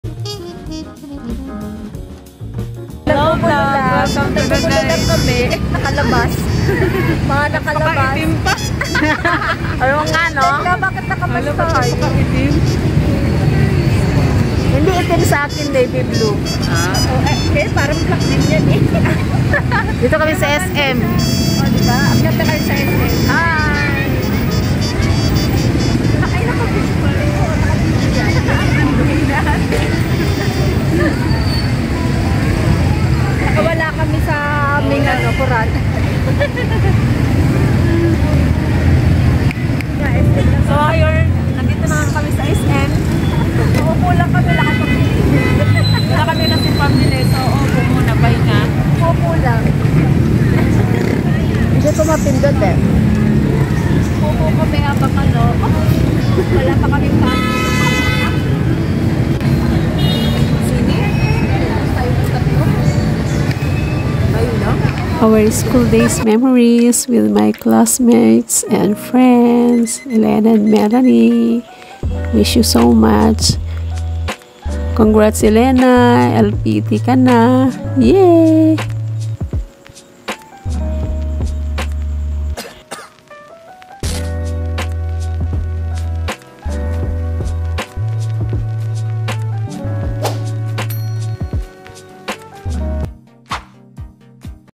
Friendship Mini Vlog
Video taken February 28, 2022. College memories with my classmates and friends. I'm missing our company! Hey, Elena, congrats on being an LPT!